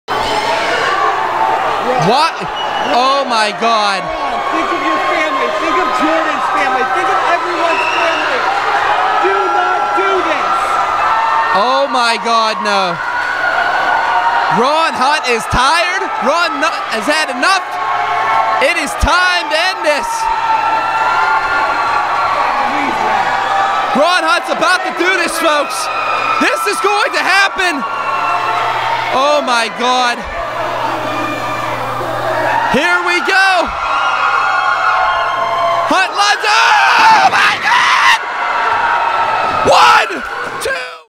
The Rev Ron Hunt's Leap of Faith
The hair raising moment from RWA PRO- Renegade Wrestling Alliance Saturday Night when The Rev" Ron Hunt took his hatred for Successful Events to the limit!